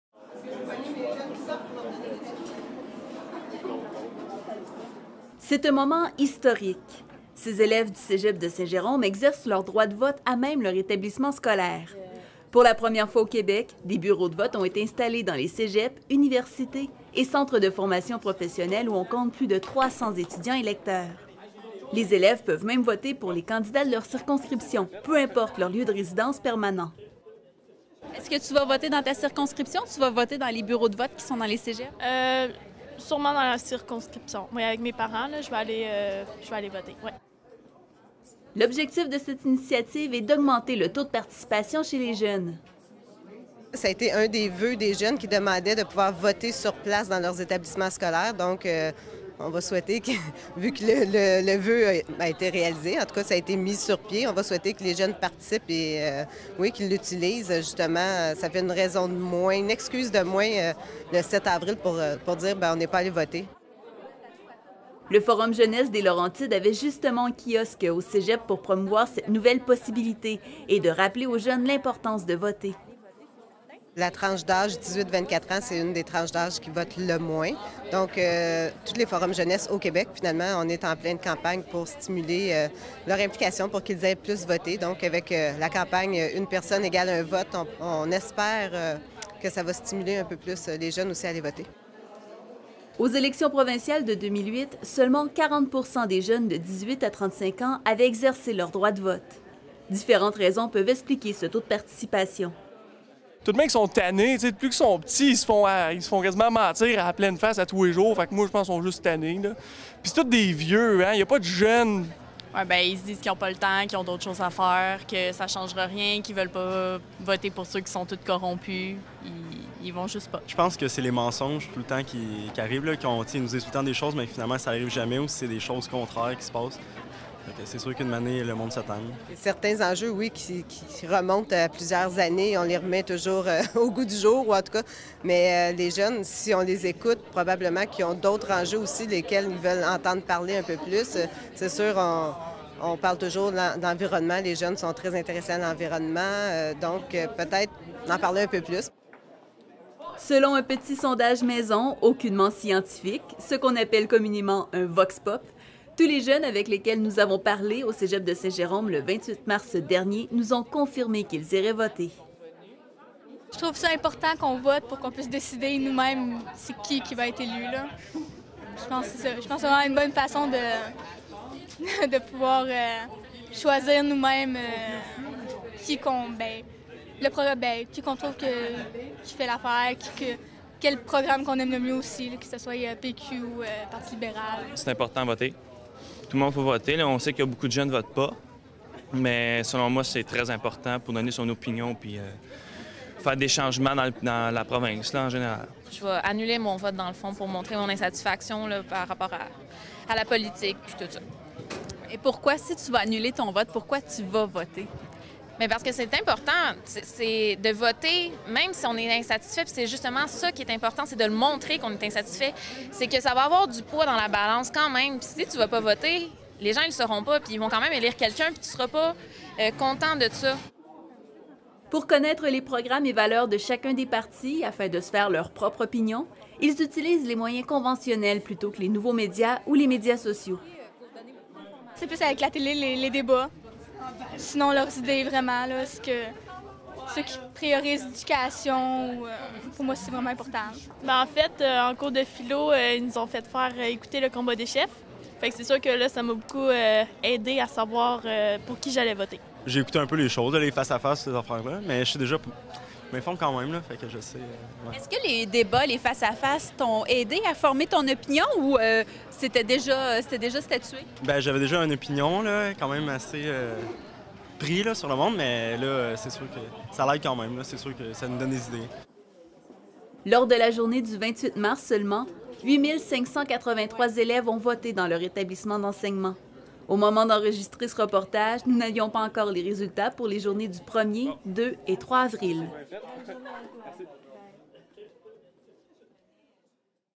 Voter au cégep
Le taux de participation des jeunes aux élections n'a jamais été très élevé. Aux élections de 2008, seulement 40 % des jeunes de 18 à 35 ans avaient votés. Pour augmenté ce taux, des bureaux de vote par anticipation ont été installés dans certains cégeps, universités et centres de formation professionnel pour que les jeunes qui n'étudient pas dans leur circonscription puissent voter.